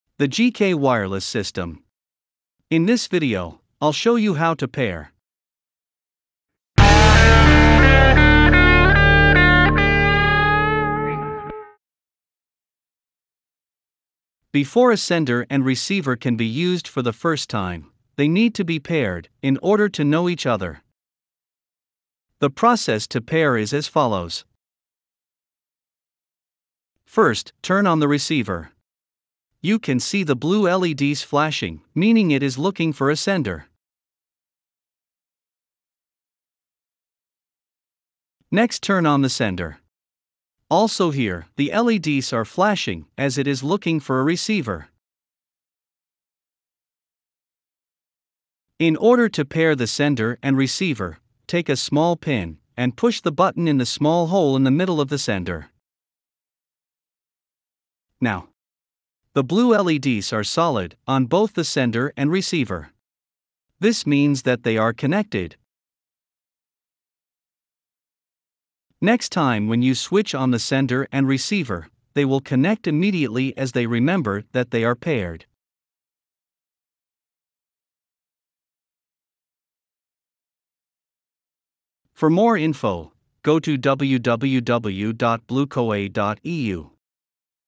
4 The GK Wireless System. Pairing
The GK Wireless System. In this video I will show you how to pair the sender and receiver.

What is the GK Wireless system? The GK Wireless System is a wireless cable solution for use with a divided pickup from Roland(GK-2, GK-3) or Godin (SA). The GKW system consists of a sender- and receiver unit that can replace the 13 pin DIN cable between a guitar and an effect unit. It is capable of wireless transmission of 6 divided pickup outputs, regular pickup output and control signals; up, down, volume and mode switch. Hence, it will give more freedom of movement to a guitar player who wants to use a guitar synthesizer or -modeler (Roland GR-55, VG-88, VG-99). It is also more reliable than a 13-pin DIN cable as these cables tend to be heavy and prone to bad connections. It has not been tested with the new Boss SY-1000 modeler, but it is expected to work as it also uses the GK-3 divided pickup.

What you see in this video is just my proof of concept. I will turn this into a commercial product when there are enough prospective buyers. The more buyers, the lower the production cost and sales price.